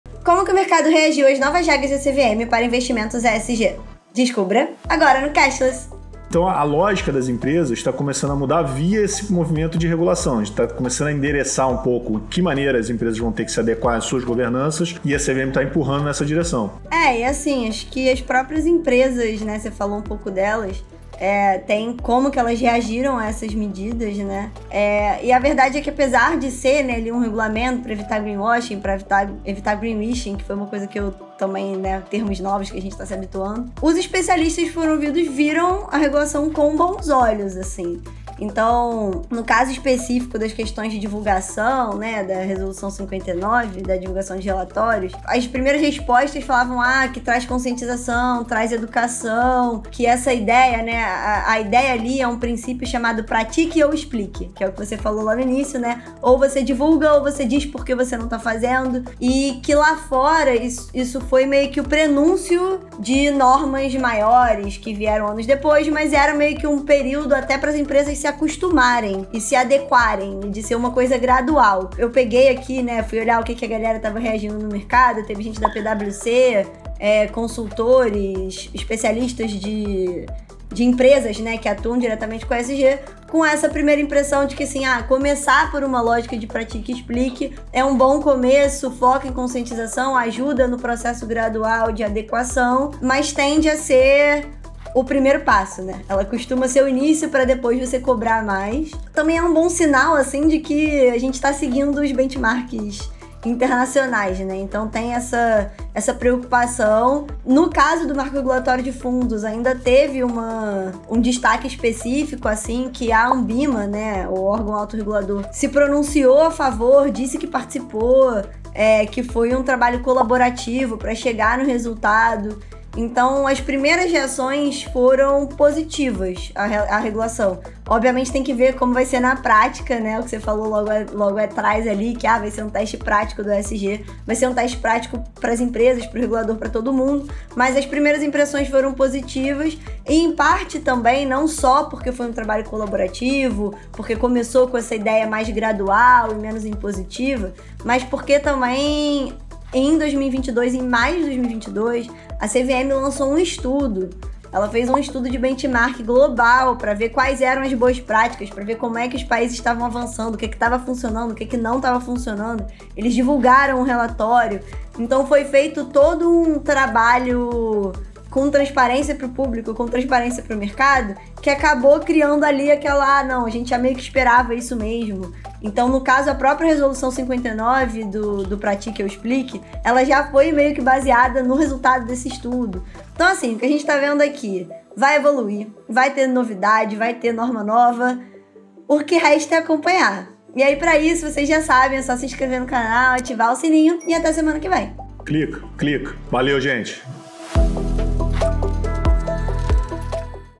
Nova regulação de ESG: quais serão os benefícios?
A pauta ESG vem ganhando espaço no mundo e no Brasil não é diferente, e nesse viés CVM impulsiona ainda mais as práticas sustentáveis e o combate de greenwashing nas empresas.

A nova resolução, aprovada no final de 2022 e com previsão para já começar esse ano, busca incentivar as companhias abertas brasileiras a adotarem práticas de ESG e a se tornarem mais transparentes.

Assista para saber mais!

Links:
O que é greenwashing? Saiba como identificar e evitar